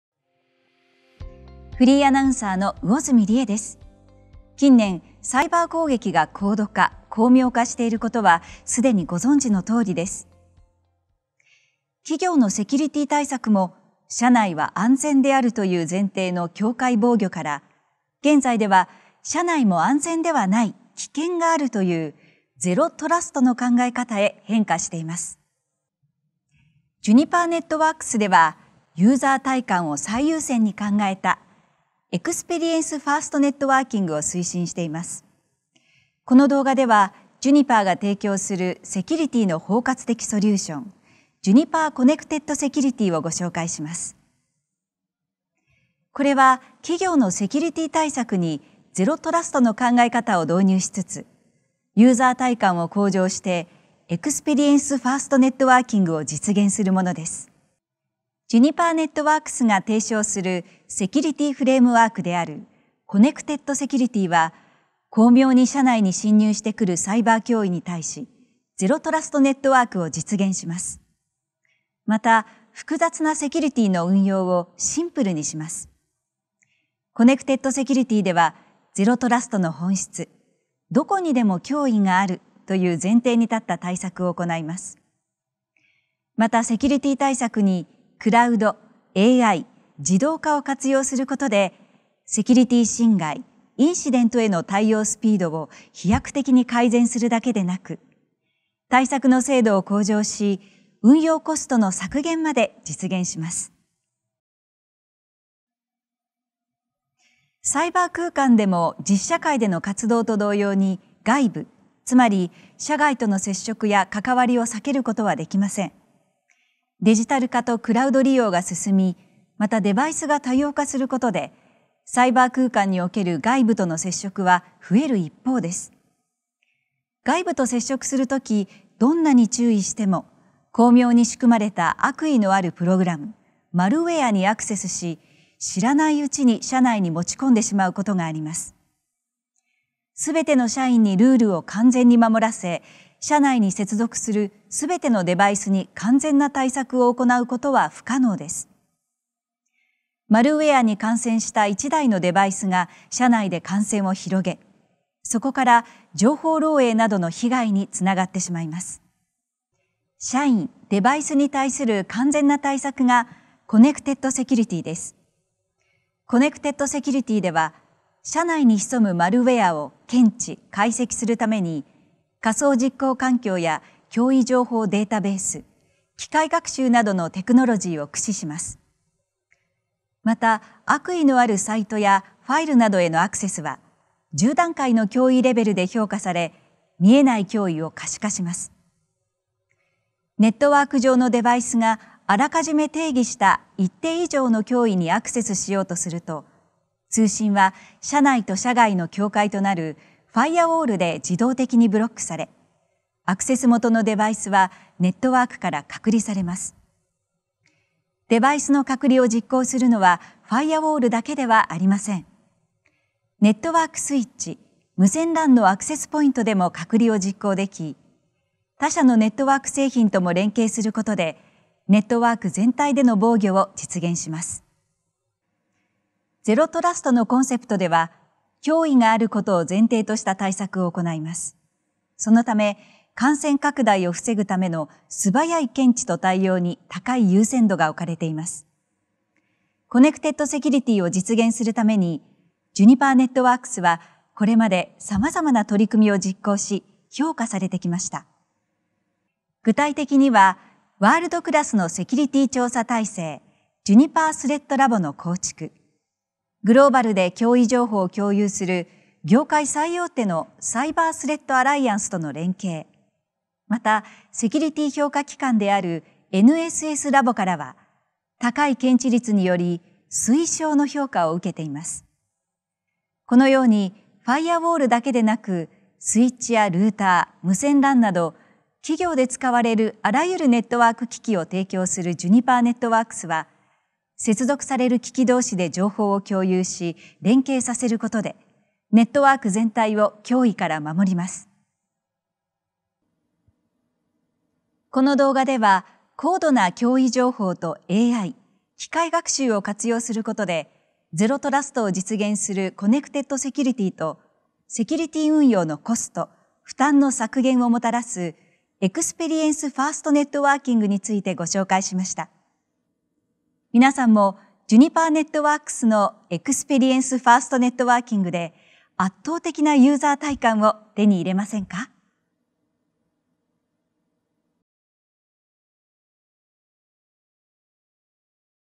ゼロトラストセキュリティのご紹介動画
「脅威はどこにでもある」を前提としたネットワークセキュリティはどのように構築すれば良いのでしょうか？　答えはジュニパーの「コネクテッド セキュリティ」にあります